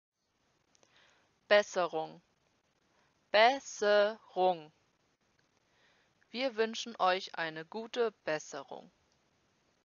Korrekten Aussprache: Bescherung
Die korrekten Aussprache von komplizierten weihnachtlichen und silvestrigen A-griffen. Nie wieder an Weihnachten blamieren, indem man Dinge falsch ausspricht! Mit für die Festtage passenden Beispiels Atzen.